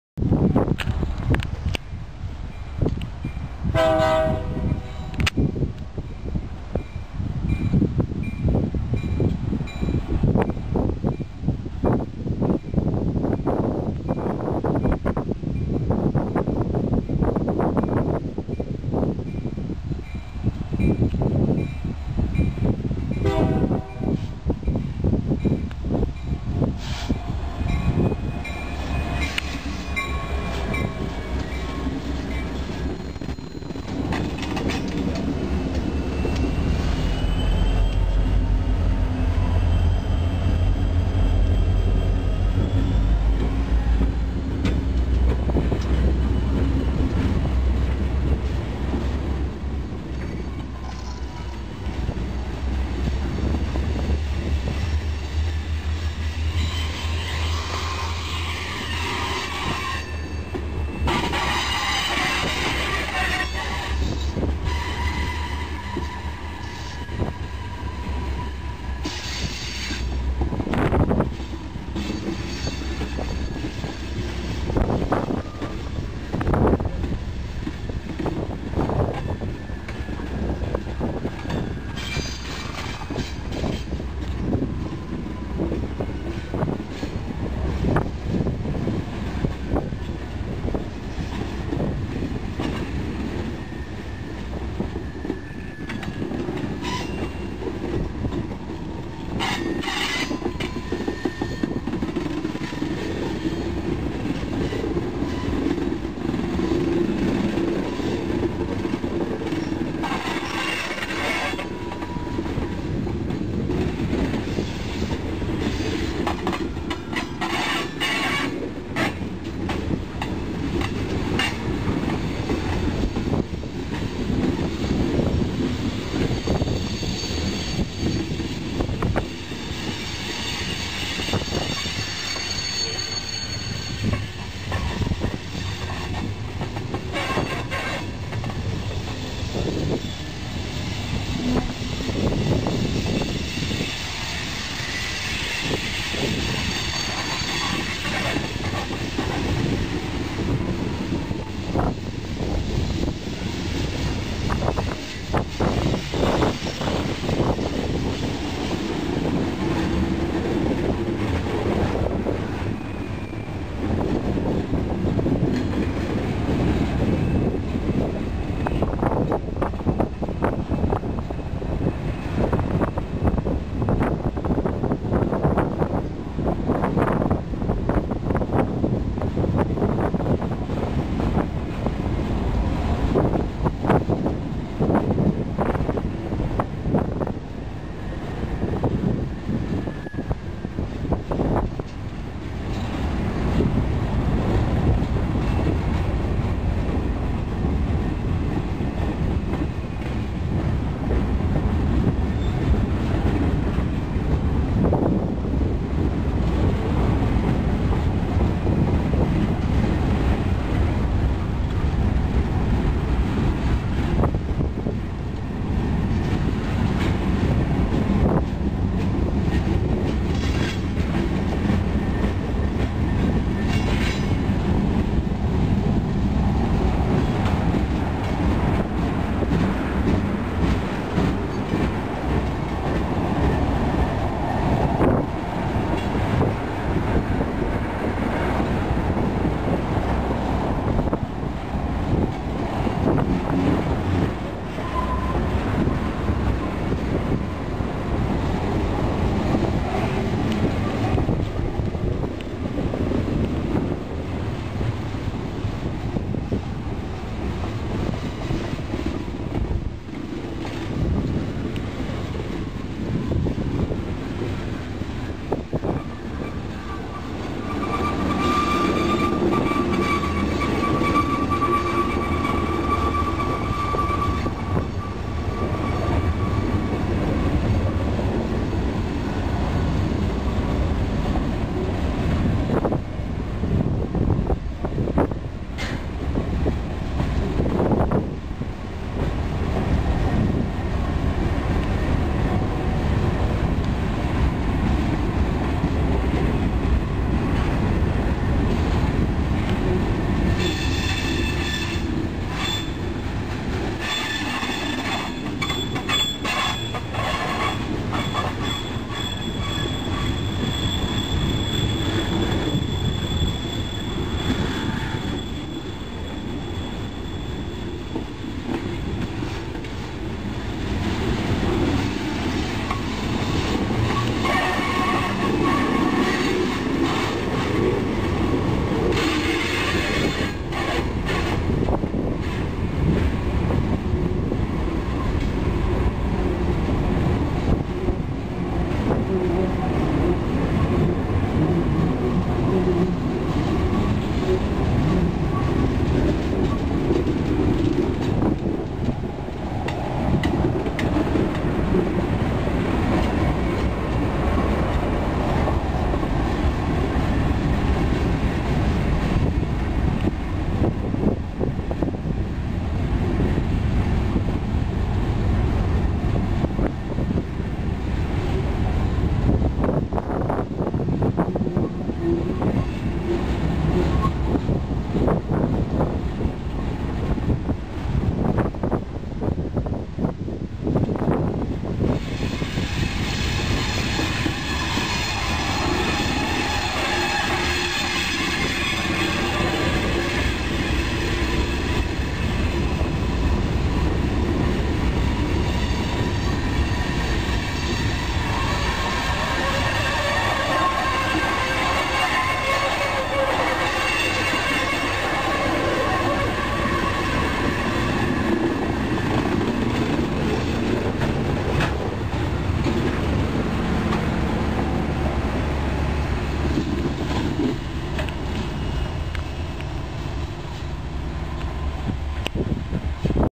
A UP Freight at Niles Junction
A Union Pacific Freight moves through Niles Junction towards Niles Canyon, Livermore, and Stockton w/ SD70M #3999, UPY ex-MP MP15DC #1342, SD70M's #4421, and #4895, and/ GP38-2 #559